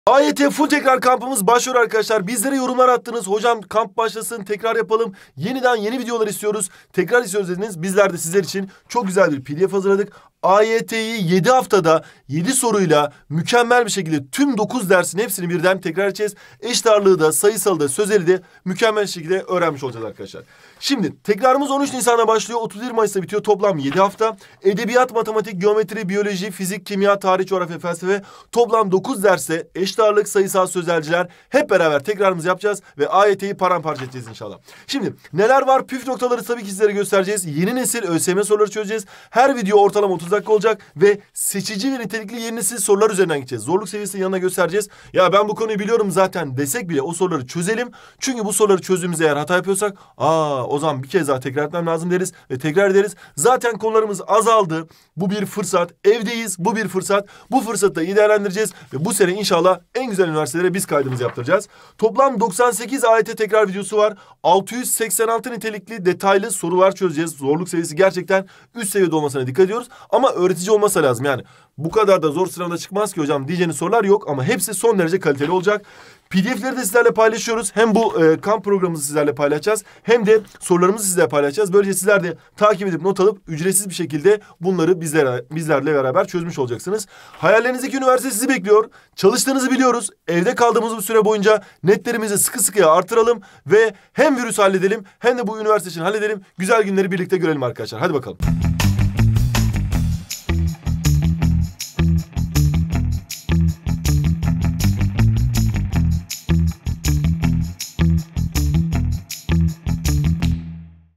7 Haftada AYT’yi Full Tekrar Ediyoruz!
AYT Full Tekrar Kampı Başlıyor! AYT'yi 7 haftada tekrar ediyoruz! 13 Nisan'da başlayan FullTekrarKampı ile tüm dersleri sizinle beraber yeni nesil sorular ve püf noktalarla tekrar ediyoruz.